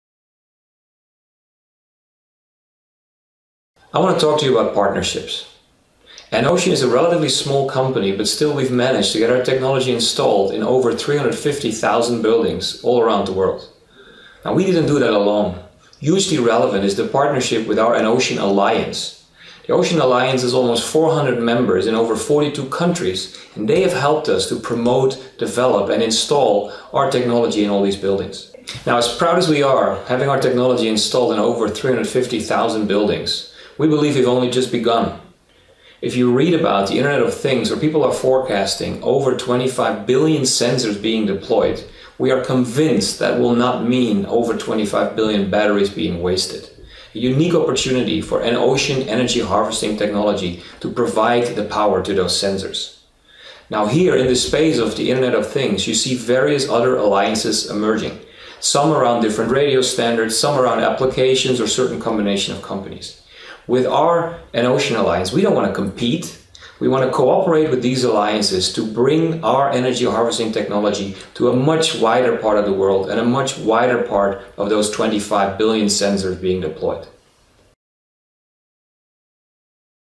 The Importance of Partnerships - from Building Automation to IoT
Dr. Wald Siskens, CEO of EnOcean, explains the importance of partnerships for EnOcean - from Building Automation to IoT.